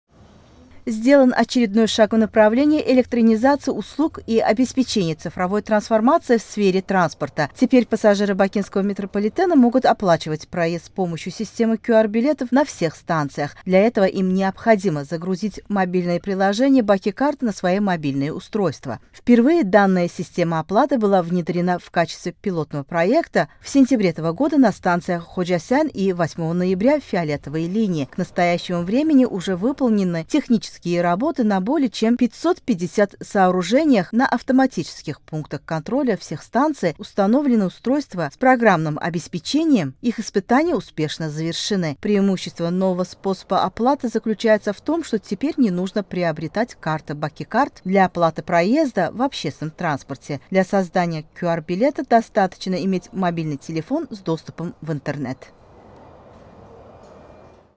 Использовать QR-билеты теперь можно на всех станциях Бакинского метро
Сделан очередной шаг в направлении электронизации услуг и обеспечения цифровой трансформации в сфере транспорта. Теперь пассажиры Бакинского метрополитена могут оплачивать проезд с помощью системы QR-билетов на всех станциях. Для этого им необходимо загрузить мобильное приложение BakiKart на свои мобильные устройства.